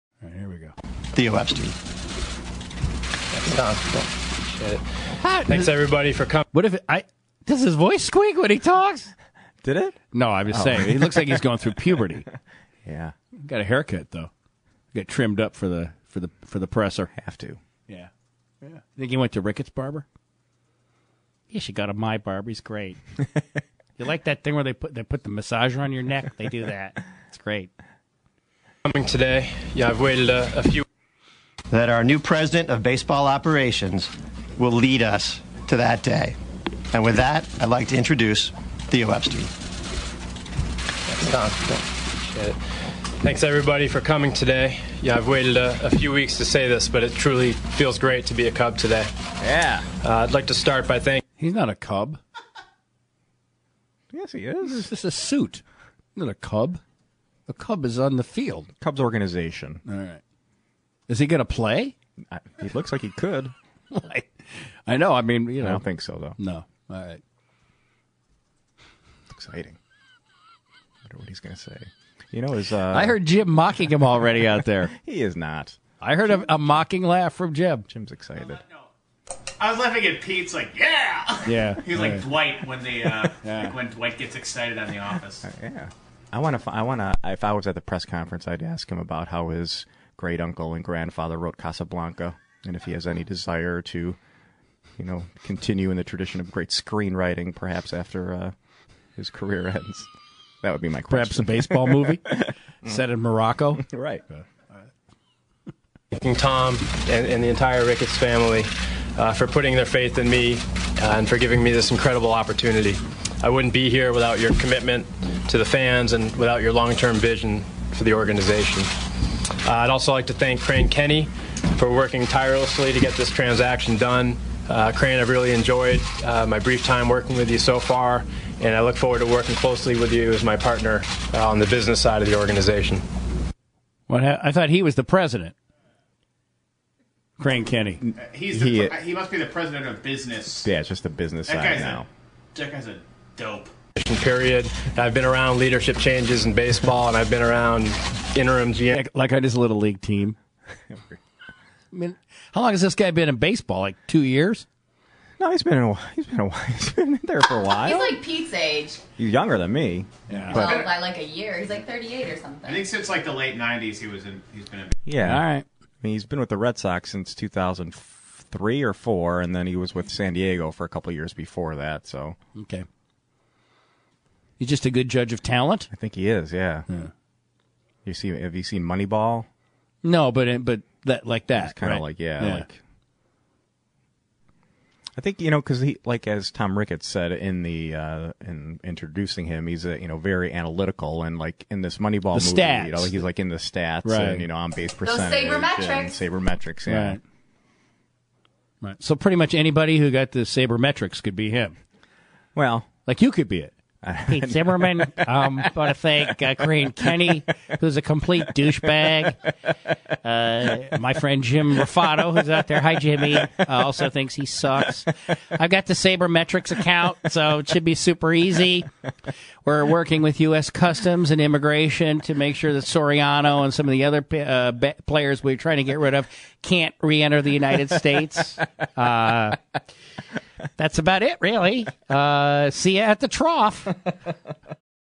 Epstein Press Conference
Theo Epstein is now with the Cubs although Steve doesn't quite understand where this kid came from. Pete explains his baseball pedigree.